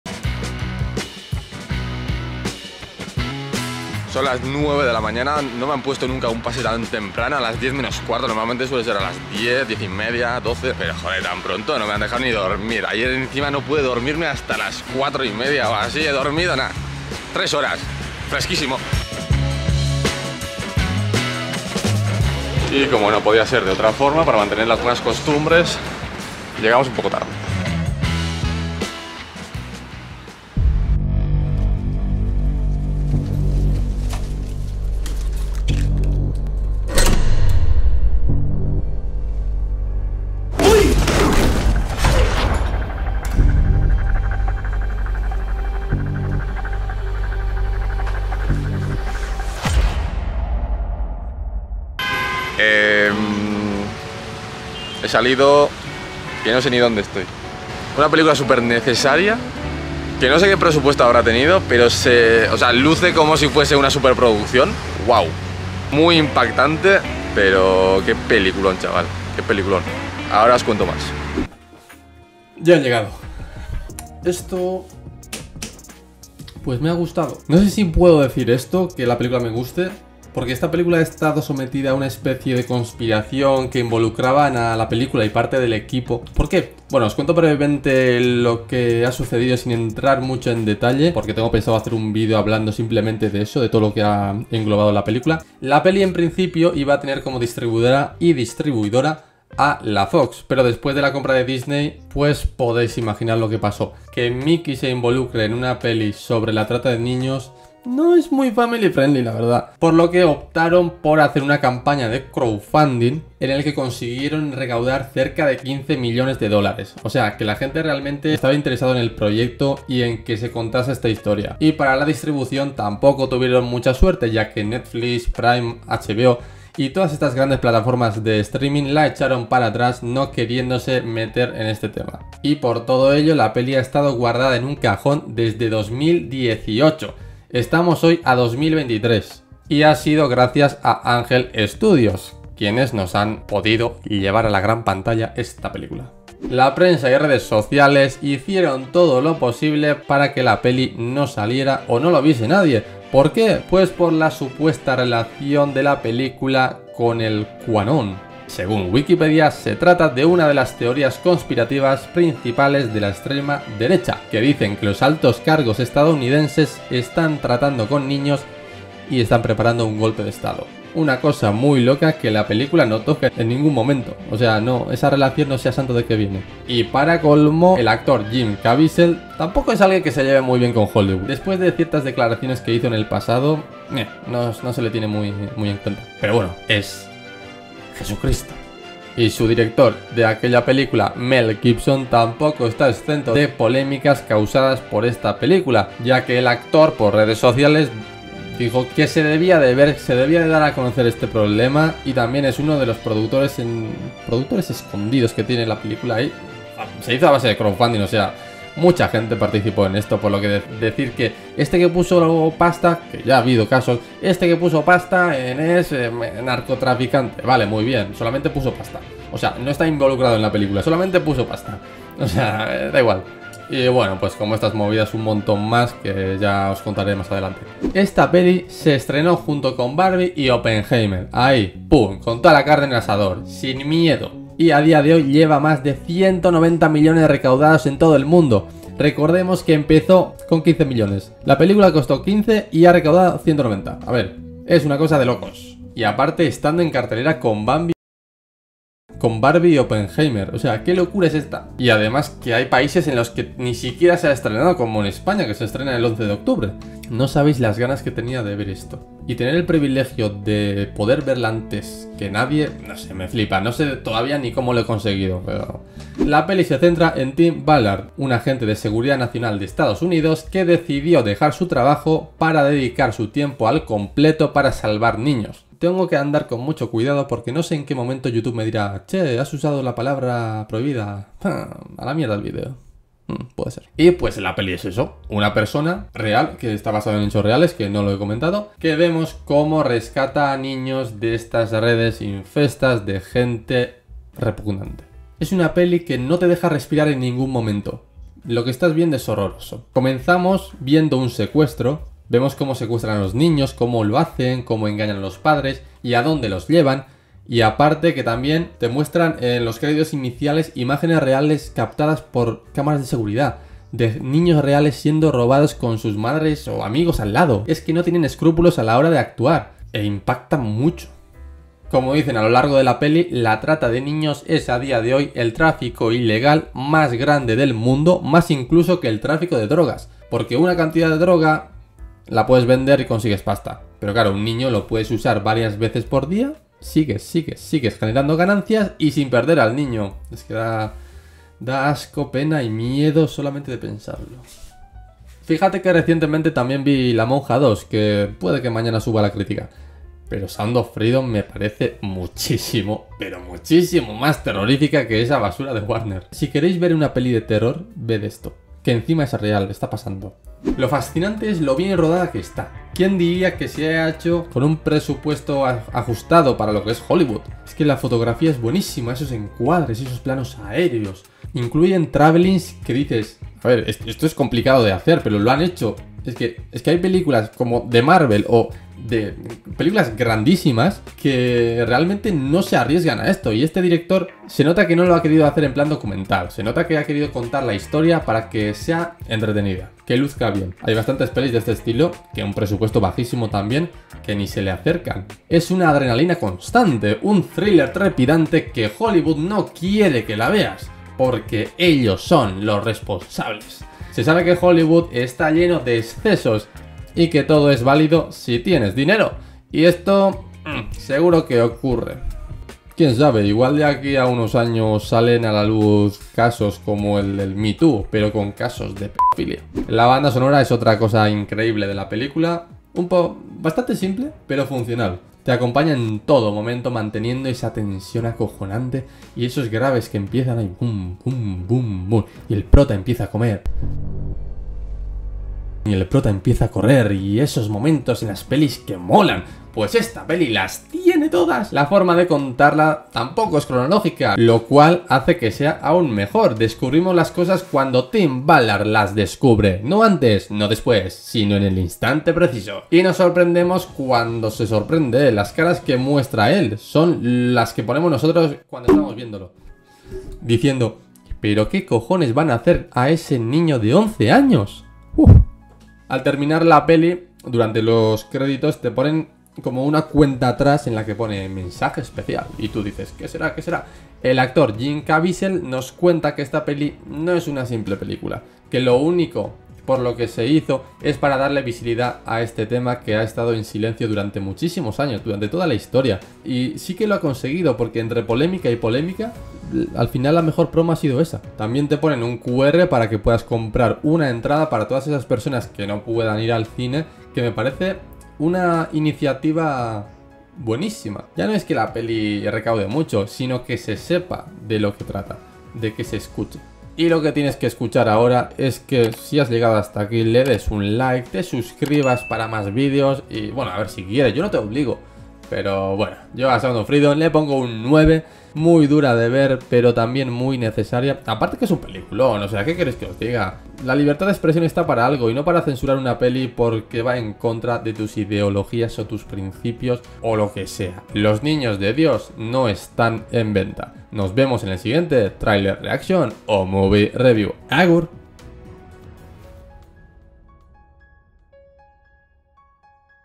La Peli que HOLLYWOOD NO QUIERE que Veas | SOUND OF FREEDOM
- Sinopsis: Narra la historia de Tim Ballard, un ex Agente de Seguridad Nacional de Estados Unidos que dejó su trabajo para dedicar su vida, sumergiéndose en el submundo del tráfico de personas a lo largo de Latinoamérica, a intentar salvar las vidas de cientos de niños.

- Reparto: Jim Caviezel, Mira Sorvino, Kurt Fuller, Bill Camp, Scott Haze, José Zúñiga, Eduardo Verastegui, Gustavo Sánchez Parra, Manny Perez, Gary Basaraba